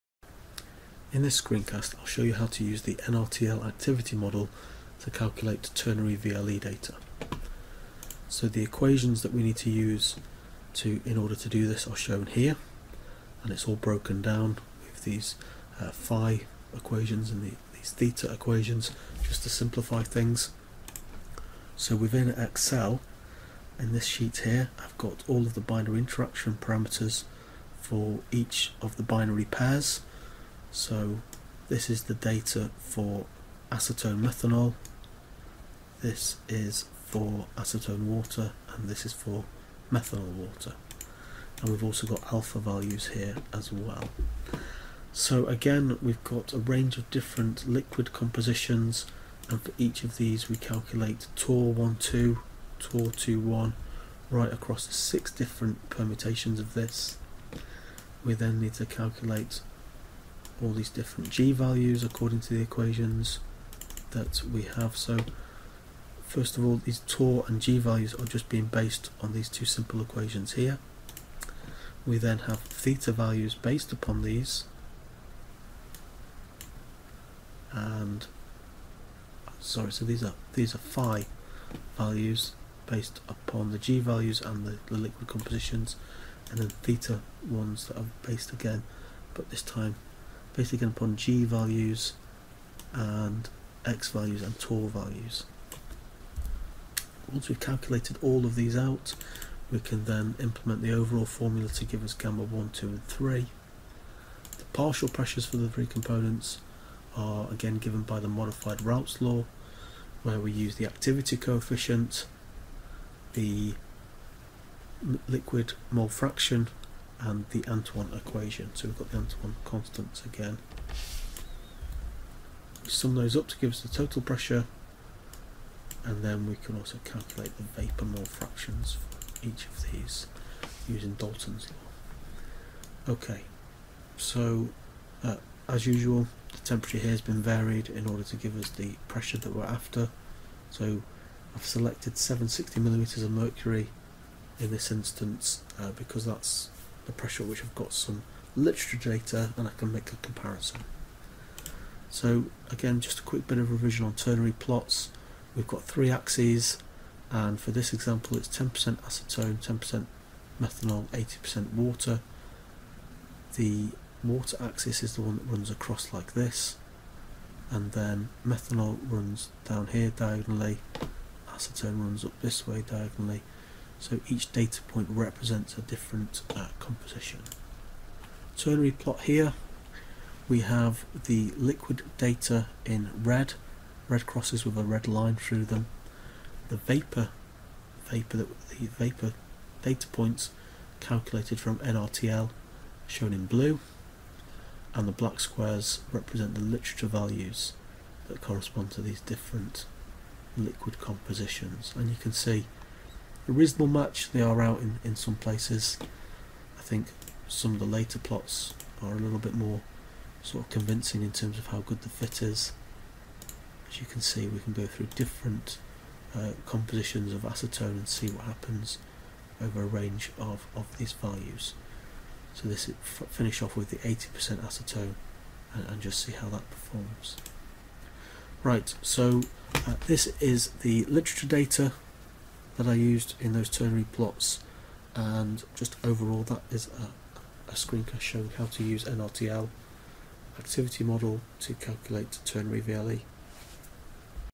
Ternary VLE calculations part 2: using the NRTL activity model for non-ideal systems
Screencast showing equations to calculate VLE for non-ideal systems and how to implement them in Excel. Uses the NRTL (non-random two-liquid) activity model. Includes ternary plots and comparison with literature values. System used to demonstrate model is acetone/methanol/water.